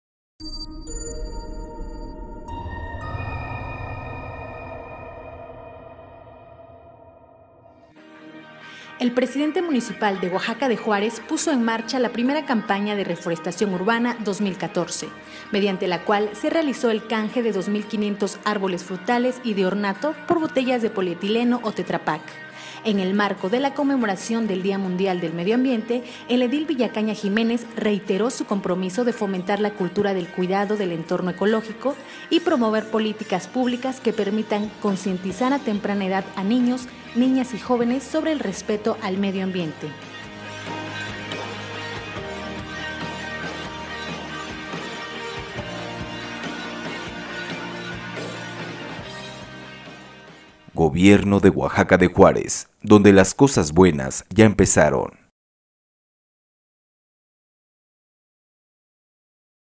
Promueve Gobierno capitalino cultura de cuidado al medio ambiente
Promueve Gobierno de la Ciudad cultura de cuidado al medio ambiente

 *Se entregaron en esta primera campaña 2 mil 500 árboles nativos de la capital del estado.

Para fomentar entre los oaxaqueños la cultura del respeto y conservación del medio ambiente, el Presidente Municipal de Oaxaca de Juárez puso en marcha la  "Primera Campaña de Reforestación Urbana 2014", en la que se realizó el canje de 2 mil 500 árboles frutales y de ornato por  botellas de Polietileno Tereftalato (PET) o  tetrapack.

Acompañado de la Directora General del Instituto Estatal de Ecología y Desarrollo Sustentable del gobierno del estado, Helena Iturribarría Rojas y del agente de Candiani, Braulio Gines Lucas, el edil capitalino manifestó que  el Gobierno de la Ciudad deja de manifiesto su interés por contribuir a la construcción de un municipio sustentable, en el que se privilegie el equilibrio ecológico y la cultura del cuidado del medio ambiente.

En conmemoración del Día Mundial del Medio Ambiente, celebrado el paso 5 del mes del curso, autoridades estatales y capitalinas iniciaron la Campaña de Reforestación Urbana 2014, que contribuye a promover entre los habitantes de los barrios, colonias y agencias de Oaxaca de Juárez, el respeto y conservación del entorno natural.

Villacaña Jiménez resaltó la importancia de concientizar a temprana edad a niños, niñas y jóvenes mediante la "Primera Campaña de Reforestación Urbana 2014", "pues serán quienes, en un futuro no muy lejano, procuren  dar continuidad a las políticas públicas en apoyo a la ecología y la conservación de un entorno saludable para el desarrollo de todos los oaxaqueños".

En su oportunidad, la directora de Ecología, Iliana Juárez Flores, destacó que el objetivo principal de la reforestación urbana es disminuir los efectos de la contaminación y fomentar la cultura del reciclaje y reutilización de productos plásticos, que por su composición son difíciles de degradar naturalmente y llegan a ocasionar diversos problemas de contaminación que afectan la salud pública.

La campaña se llevó a cabo a un costado de la explanada del Centro Monitoreo Ambiental "Casa de la Tierra", ubicado en la Agencia Municipal de Candiani, hasta donde cientos de ciudadanos llevaron botellas de PET y de tetrapack, para ser canjeados por un árbol nativo de la capital.

En la jornada se entregaron más de 2 mil 500 ejemplares arbóreos que se entregarán en esta "Primera Campaña de Reforestación 2014" se encuentran especies frutales y de ornato como flor de mayo, macuil, pochote, mandarinos, naranjales, nanches, nísperos, bugambilias y huajes.

Participaron en esta campaña vecinos de la capital del estado y representantes de organizaciones públicas y privadas, así como de diversos centros educativos de nivel básico.

Posterior al canje de árboles, el edil de la capital, en compañía del agente de Candiani realizó un recorrido por las inmediaciones y el interior del Mercado Zonal Candiani, en donde constató diversas necesidades de los locatarios e instruyendo inmediatamente a sus colaboradores a realizar las gestiones necesarias para solucionarlas.

Por último, el munícipe capitalino se trasladó al interior Centro Monitoreo Ambiental "Casa de la Tierra" y revisó diversos mecanismos de proyección con los que cuenta el inmueble para promover el cuidado del medio ambiente entre los oaxaqueños.

A la campaña de reforestación asistieron también el secretario de Desarrollo Humano, Leandro Hernández García y el Secretario de Desarrollo Urbano, Ecología y Obras Públicas, Eduardo Narváez Wilson.